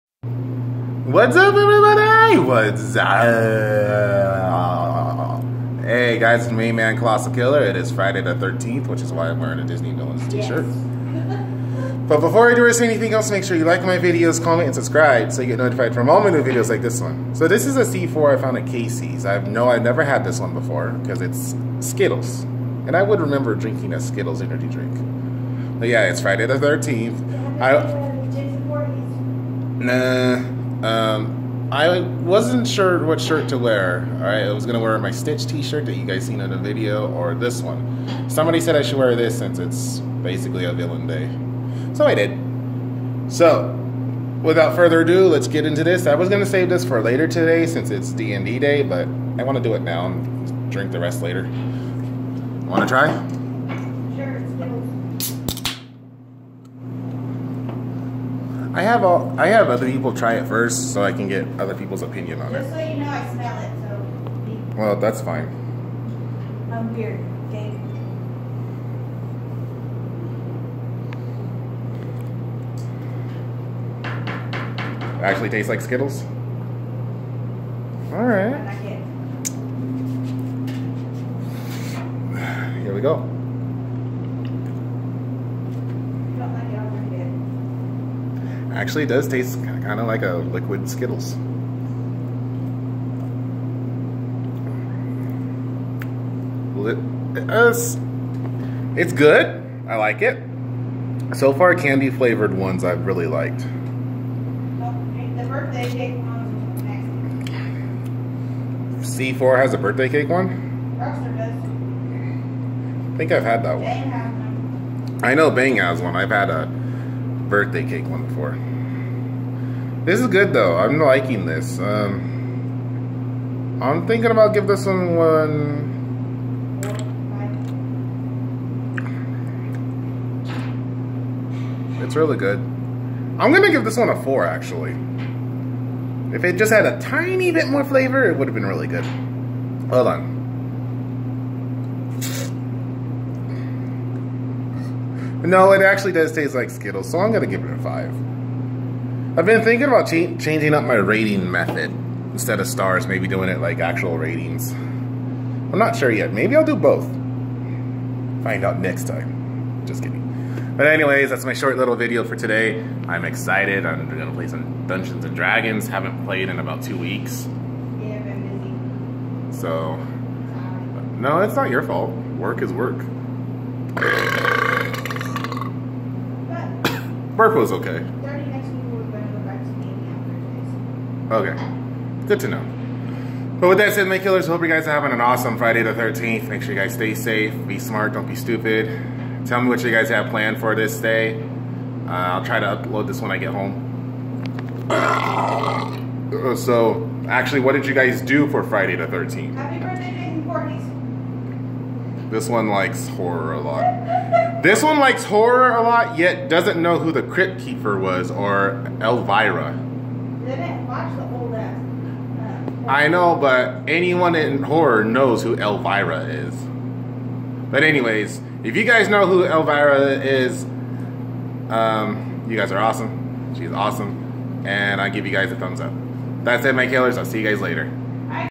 reviews C4 energy Skittles
I can't believe he didn't know who Elvira was you can't not like horror and not know who Elvira is anyway this drink actually does taste like Skittles to me I gave it 5 stars 🌟🌟🌟🌟🌟 I almost gave it for but I changed it and give it five it's very good I recommend you guys trying if you like Skittles.